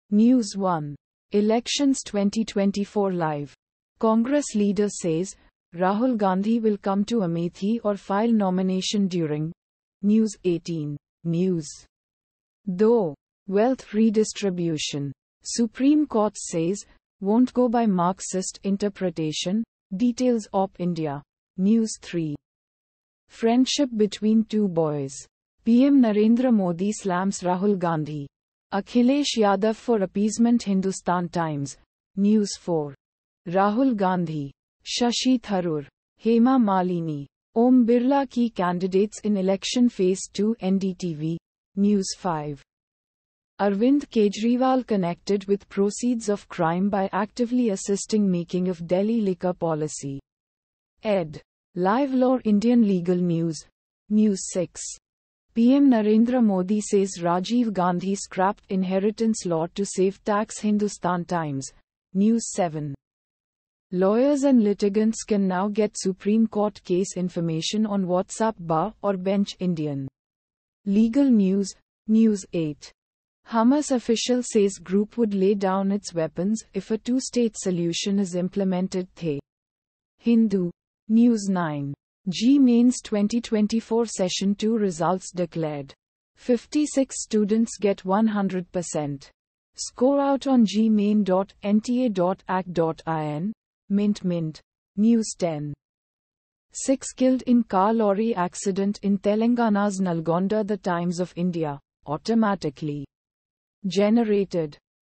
News Headlines (India)- 26/04/2024,05:58 PM GMT+5:30
News 1:Elections 2024 LIVE: Congress Leader Says 'Rahul Gandhi Will Come to Amethi & File Nomination During...' - News18
News 2:Wealth redistribution: Supreme Court says ‘won’t go by Marxist interpretation’- Details - OpIndia
News 3:'Friendship between 2 boys...': PM Narendra Modi slams Rahul Gandhi, Akhilesh Yadav for appeasement - Hindustan Times
News 4:Rahul Gandhi, Shashi Tharoor, Hema Malini, Om Birla - Key Candidates In Election Phase 2 - NDTV
News 5:Arvind Kejriwal Connected With Proceeds Of Crime By Actively Assisting Making Of Delhi Liquor Policy : ED ... - Live Law - Indian Legal News
News 6:PM Narendra Modi says Rajiv Gandhi scrapped inheritance law to save tax - Hindustan Times
News 7:Lawyers and litigants can now get Supreme Court case information on WhatsApp - Bar & Bench - Indian Legal News
News 8:Hamas official says group would lay down its weapons if a two-state solution is implemented - The Hindu
News 9:JEE Mains 2024 Session 2 results DECLARED: 56 students get 100%, score out on jeemain.nta.ac.in | Mint - Mint
News 10:Six killed in car-lorry accident in Telangana's Nalgonda - The Times of India

Automatically Generated...